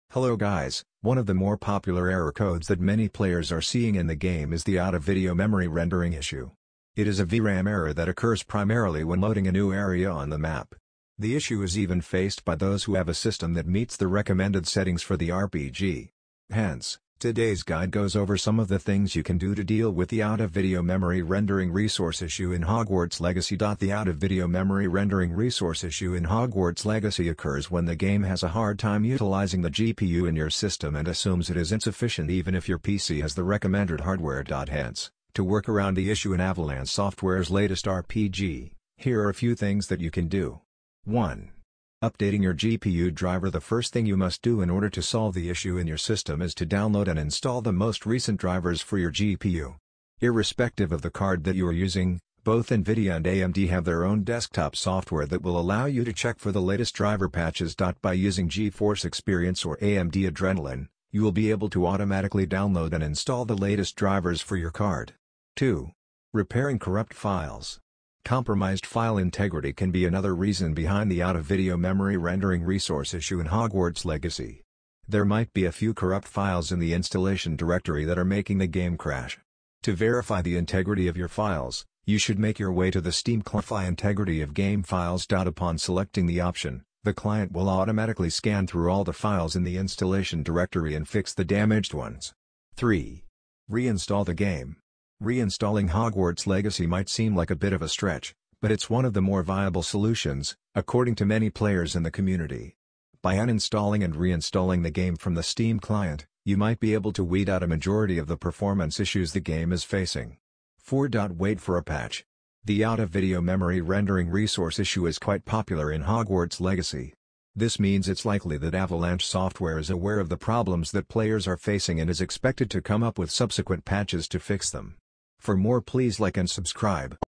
How to Fix Out of Video Memory Rendering Resource Error in Hogwarts Legacy
Out of Memory Error Fix

Hogwarts Legacy has arrived with a fair amount of performance issues, especially for PC players, who are encountering a lot of stuttering issues and game crashes.One of the more popular error codes that many players are seeing in the game is the "Out of video memory" rendering issue. It is a VRAM error that occurs primarily when loading a new area on the map. The issue is even faced by those who have a system that meets the recommended settings for the RPG.